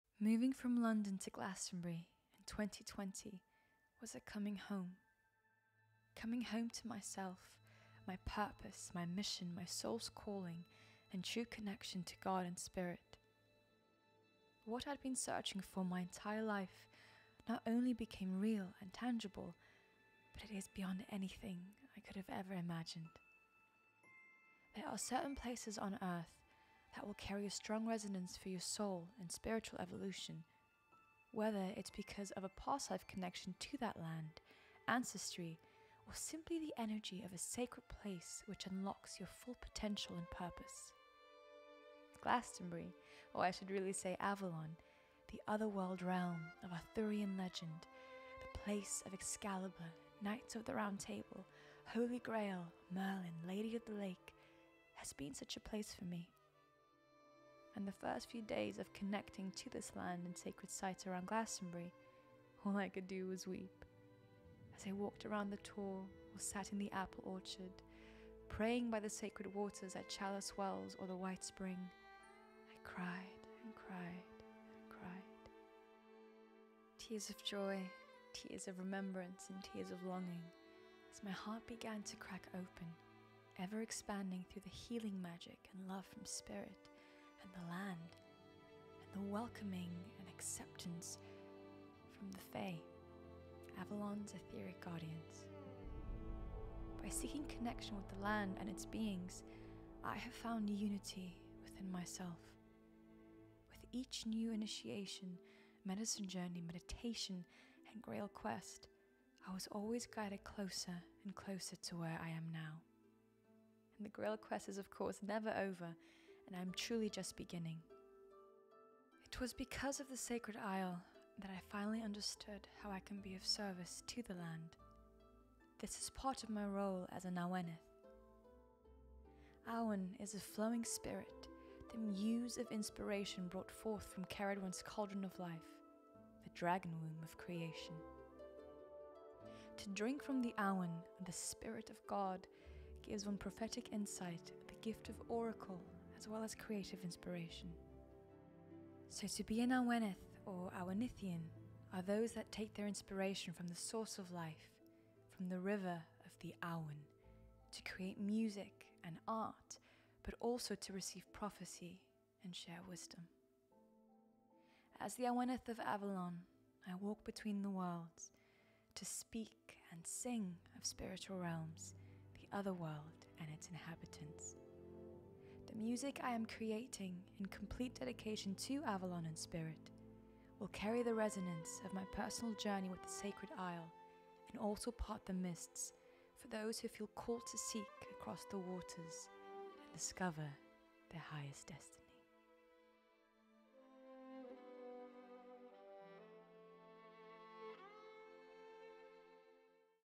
My Journey As A Priestess Of Avalon - Xiomara Crystal
To be an Awenydd, is my sacred destiny.

Awen is the flowing Spirit, the muse of inspiration brought forth from Ceridwens Cauldron of Life, the Dragon Womb of Creation.

To “drink” from the Awen, the “spirit of God” gives one prophetic insight, the gift of Oracle as well as creative inspiration.

So to be an Awenydd or Awenyddion (plural) are those that take their inspiration from the source of life, from the river of the Awen, to create music & art but also to receive prophecy & share wisdom.

As the Awenydd of Avalon, I walk between the worlds, to speak & sing of spiritual realms, the Otherworld and its inhabitants...

The music I am creating, in complete dedication to Avalon & Spirit, will carry the resonance of my personal journey with the Avalon & also part the mists for those who feel called to seek across the waters & discover their highest destiny…